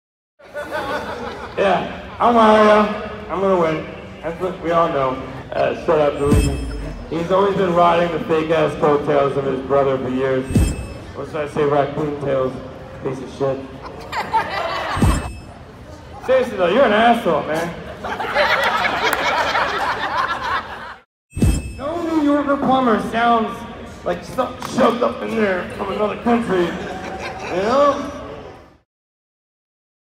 Wario - Roast Of Mario
Dais Of Thunder Roast of Mario at Thy Geekdom Con. John Rice as Wario roasts Mario.